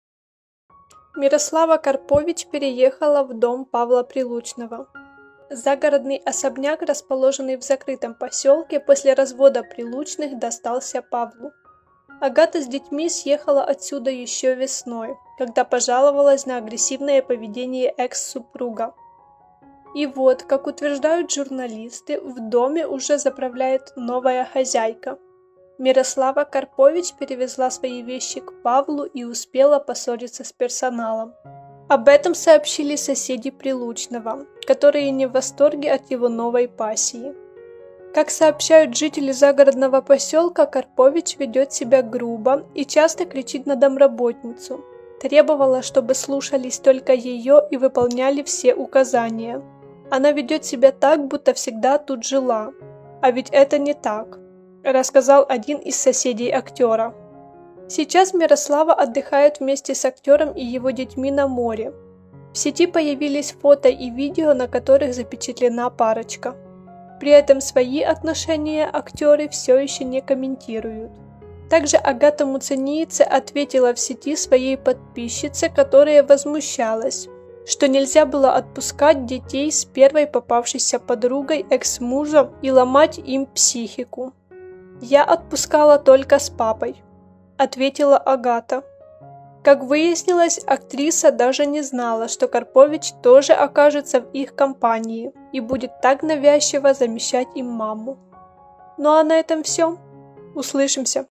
Карпович переехала в дом Прилучного | Агата Муцениеце прокомментировала отдых Прилучного с Карпович
Агата Муцениеце прокомментировала роман Павла Прилучного с Мирославой Карпович